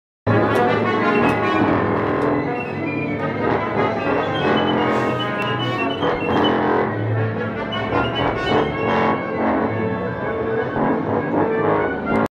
Orchestra Rehearsal
Tchaikovsky symphony orchestra rehearsal.